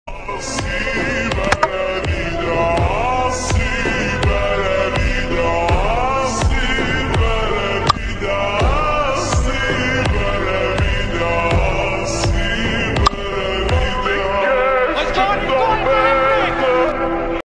no one can swinging ball like chaminda vaas🥶👿
Chaminda vaas created world record 2003 World Cup match first three ball taken by three wickets against bangladesh.@CRICKSTORY123